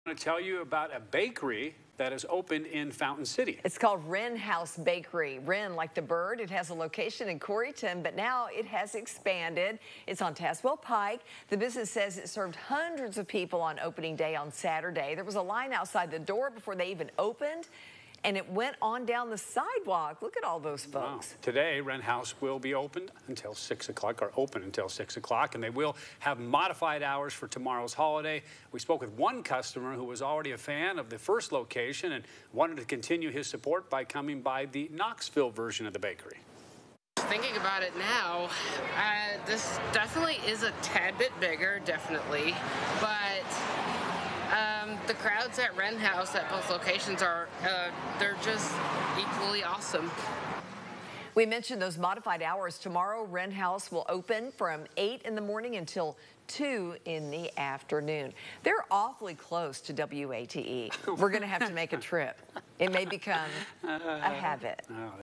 WrenHouse opens Knoxville location
A new bakery is open in Knoxville. WrenHouse opened on July 1, it's their second location with the first in Corryton.